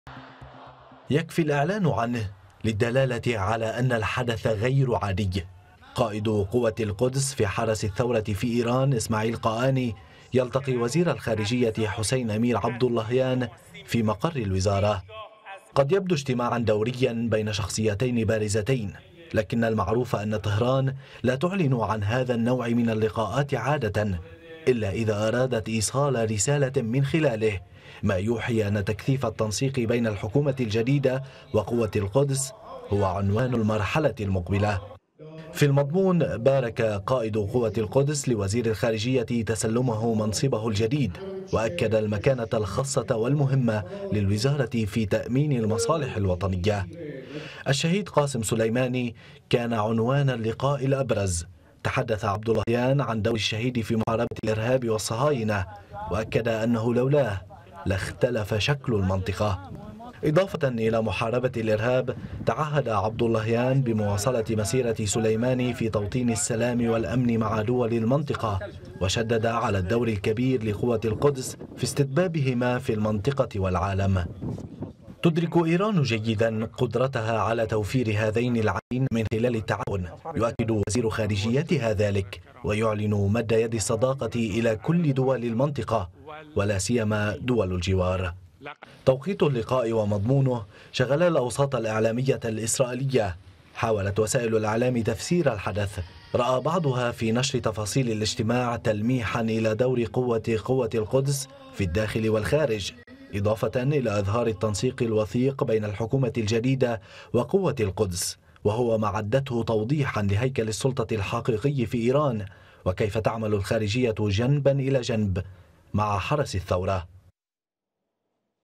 وزير الخارجية الإيرانية يستقبل قائد قوة القدس في حرس الثورة
إعداد: حسين السيد